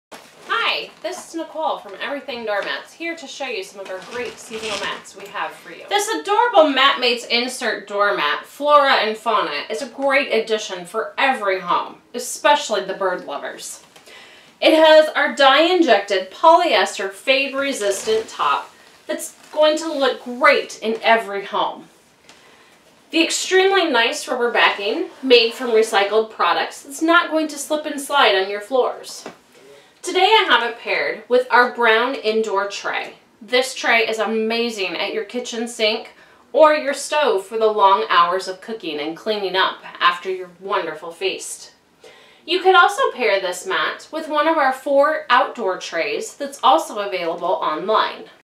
Flora & Fauna MatMate Insert Doormat - 18 x 30
To help their customers even more William & Nicole have put together a series of short videos showing off the different features of their mats. This video shows a 2016 spring doormat - Flora & Fauna MatMate Insert Doormat.